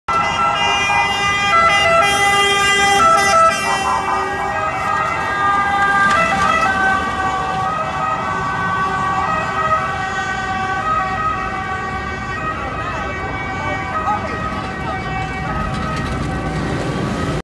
Rome, Italy - Fire trucks, hi-lo siren
Fire trucks captured 26 MAR 2019 somewhere in Rome